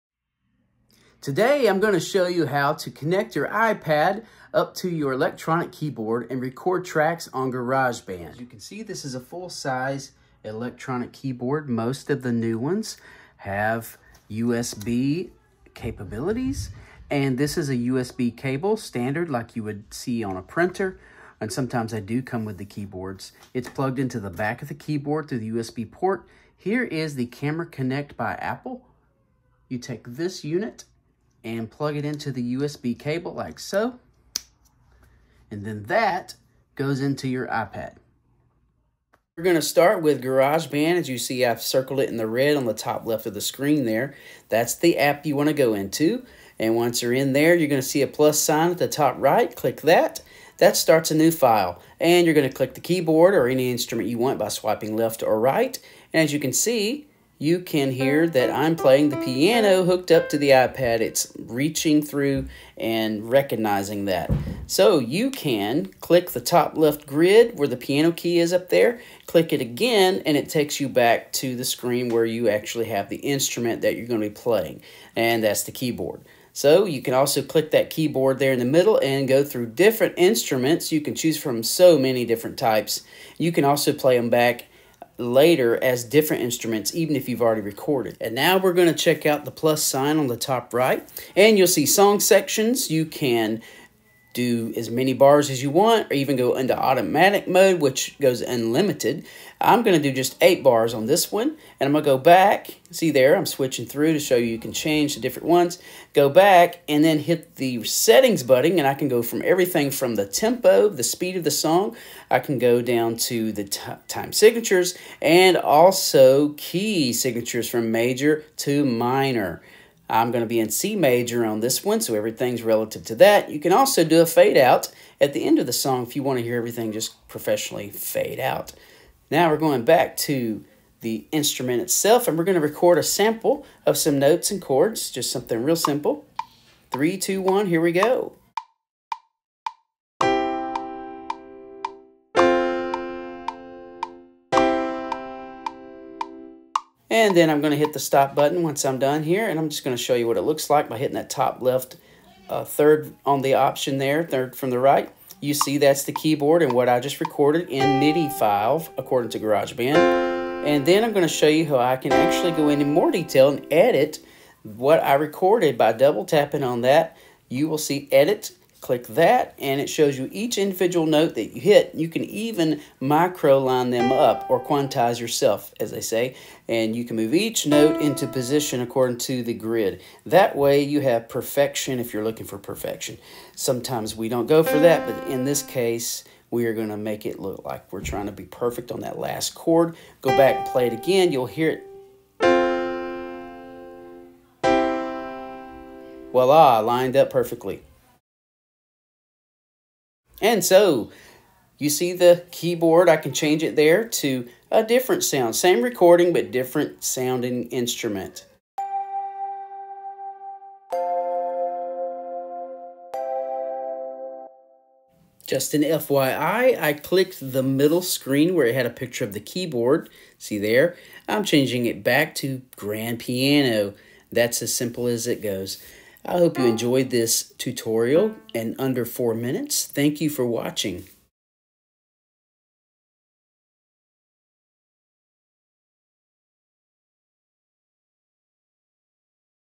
How to record an electronic piano keyboard onto GarageBand | under 4 minutes!
In this video I demonstrate in under 4 minutes how to record an electronic piano keyboard onto GarageBand. This is helpful for those attempting to share their music ideas or even songs with the world digitally.